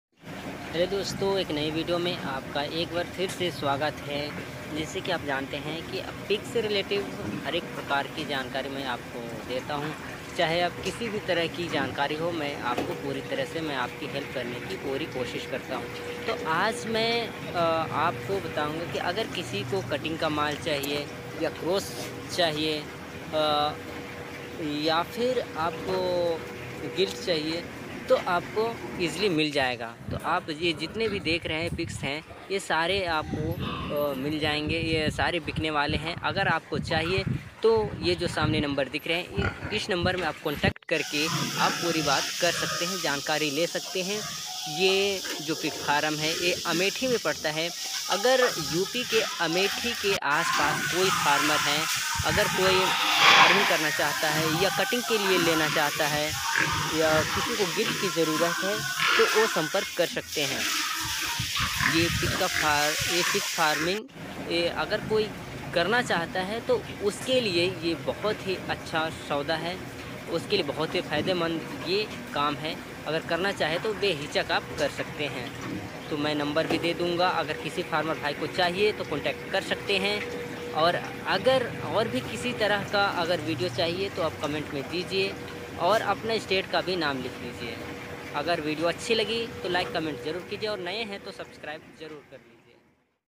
कटिंग, गिल्ट, क्रॉस पिग चाहिए तो संपर्क करें अमेठी
yah pig farm Amethi me hai yaha pura pig larz wait me kaam hota hai agar kisi ko chahiye to le sakte hai yaha cutting, ke liy palne ke liy gilts ya cross pig chahiye to mil jayegi
sampark kare